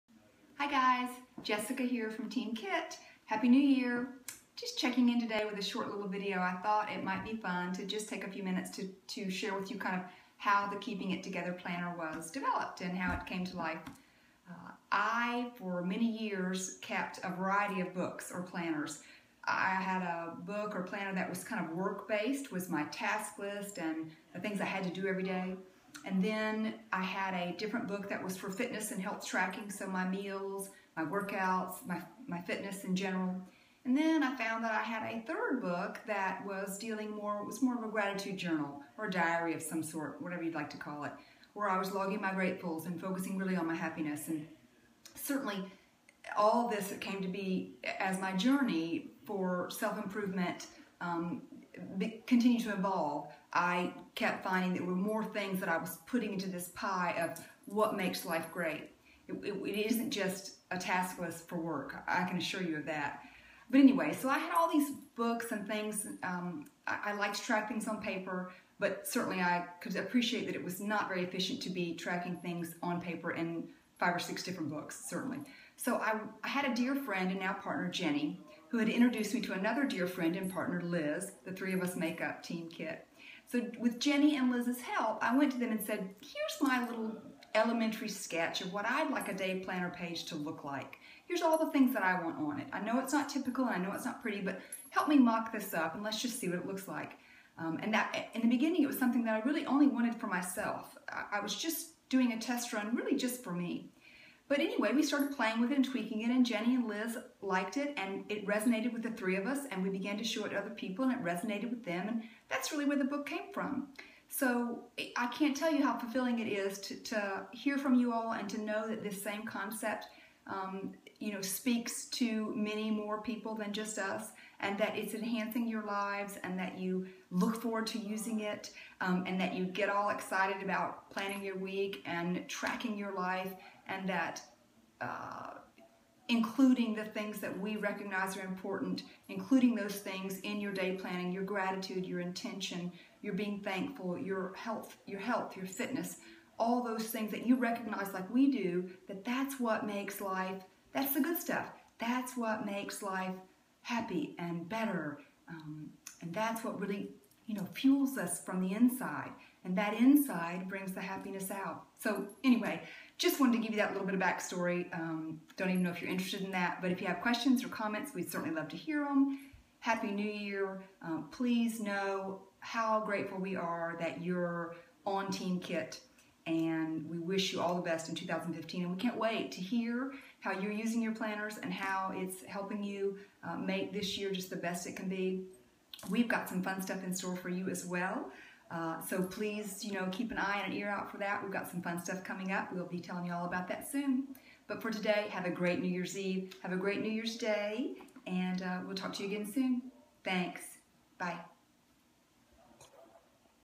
How I Use My Planner - The Kickstart of KIT - Keeping It Together Daily Planner
Our very own Jessica Locke gives a background on how kitlife.net came to be.

Wishing you all a very happy New Year's Eve and New Year! Jessica shares with viewers how the Keeping it Together Daily Planner started. The concept was born, a team was developed and the end result resonated with us and all of you! We are so fulfilled that this our daily planner speaks to each of you! We love to hear how excited you all are about planning your week and tracking your life with this planner that focuses on gratitude, intention, health and fitness.

In the video with Jessica is kitlife.net's Daily Planner for 2015 - KIT2015. Check it out on our website and shop at kitlife.net!